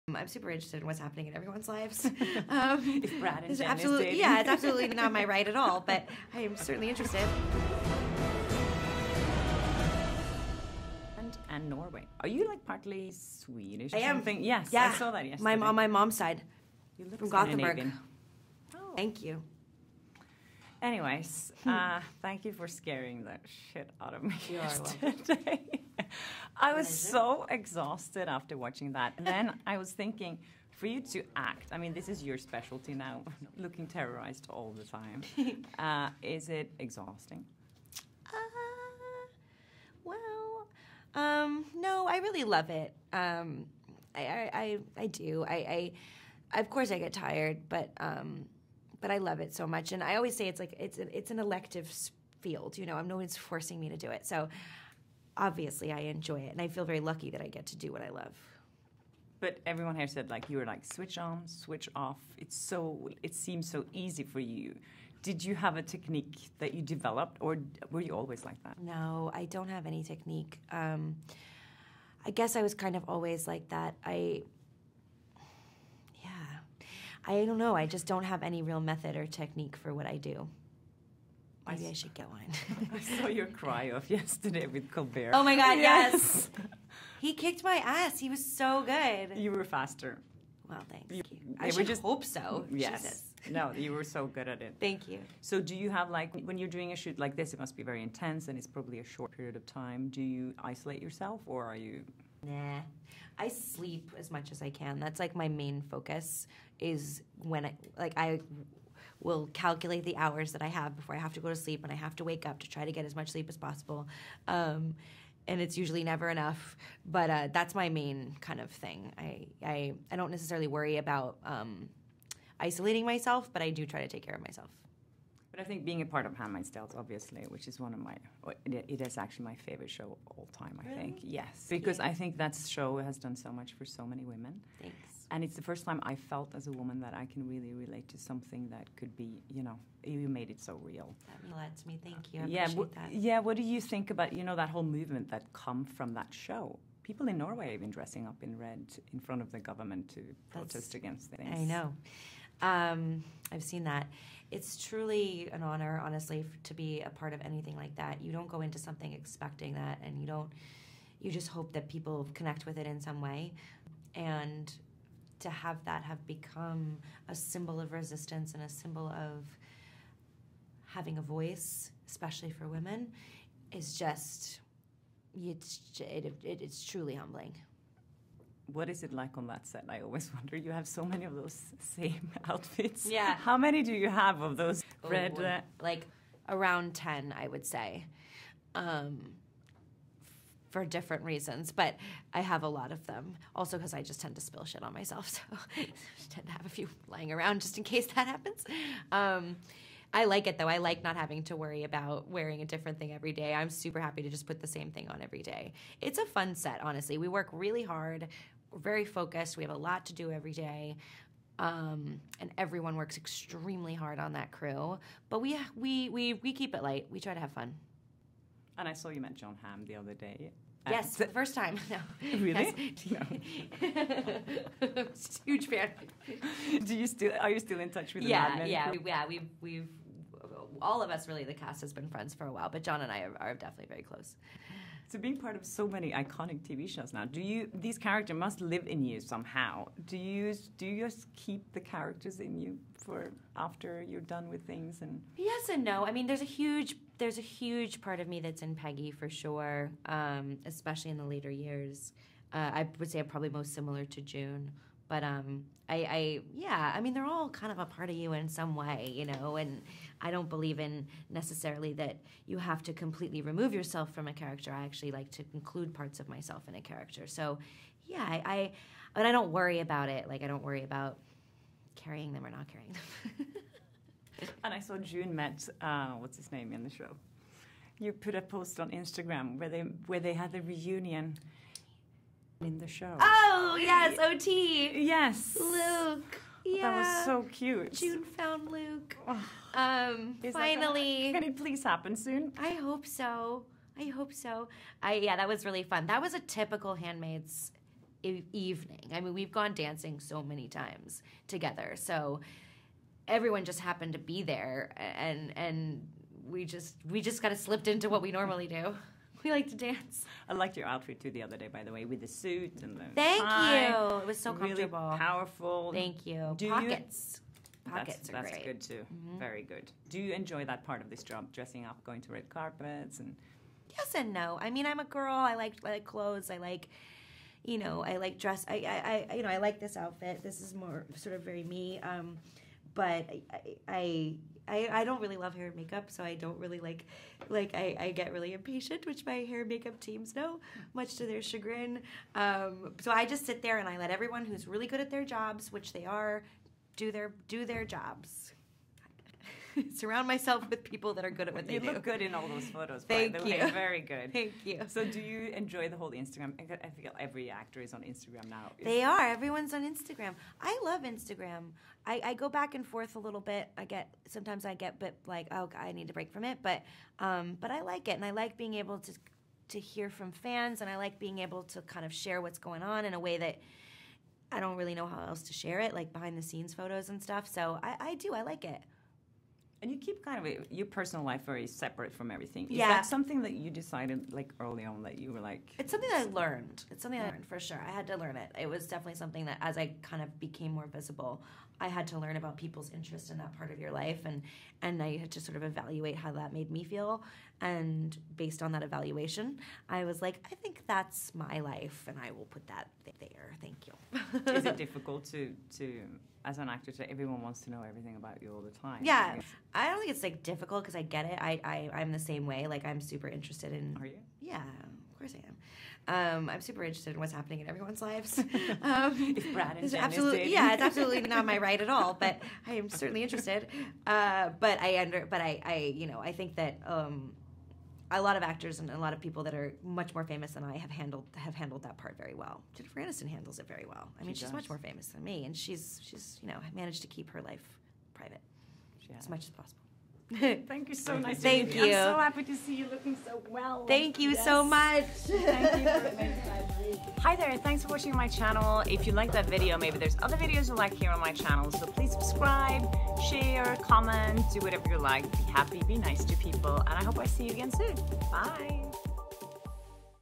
Elisabeth Moss on why she is the best to fake CRY in Hollywood ...
Elisabeth Moss talks Privacy, Jennifer Aniston, Brad, Instagram -  why she is so good at crying, how Handmaid's Tale cast loves to go dancing. How she keeps in touch with Jon Hamm (Mad Men) and how she is so interested in celebrity gossip like the rest of us.